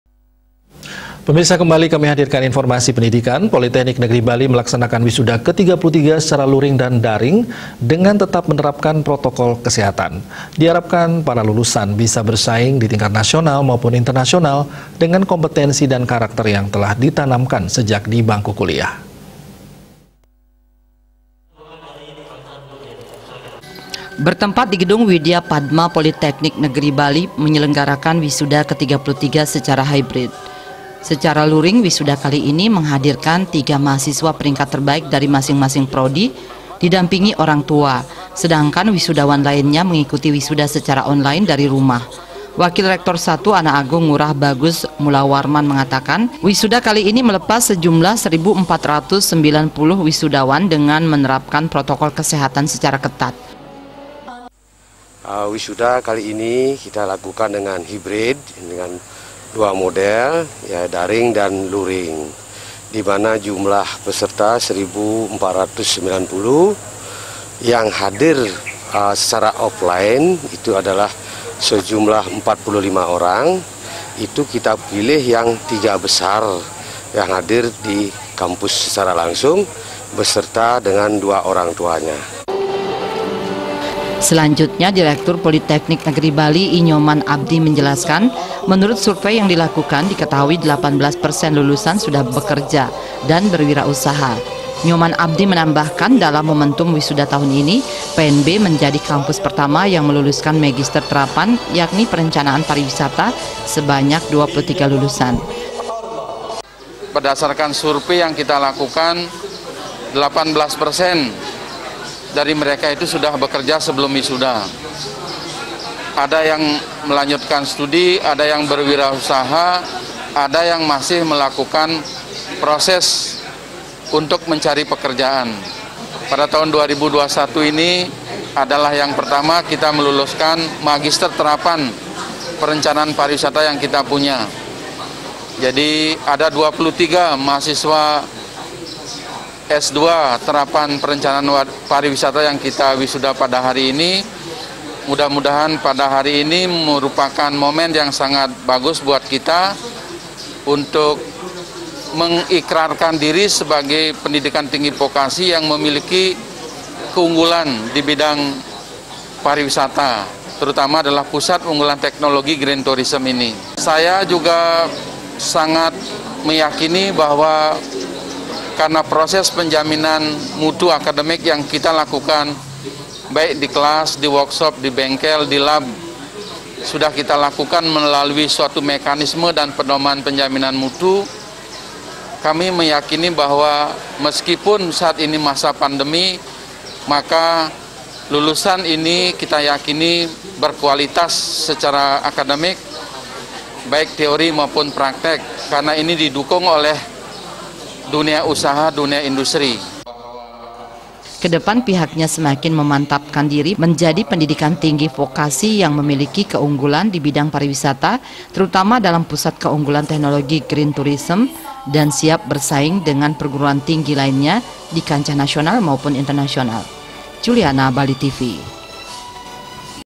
WISUDA PNB KE 33 LEPAS 1 490 WISUDAWAN
POLITEKNIK NEGERI BALI MELAKSANAKAN WISUDA KE-33 SECARA LURING DAN DARING, DENGAN TETAP MENERAPKAN PROTOKOL KESEHATAN. DIHARAPKAN, PARA LULUSAN BISA BERSAING DI TINGKAT NASIONAL MAUPUN INTERNASIONAL, DENGAN KOMPETENSI DAN KARAKTER YANG TELAH DITANAMKAN SEJAK DI BANGKU KULIAH.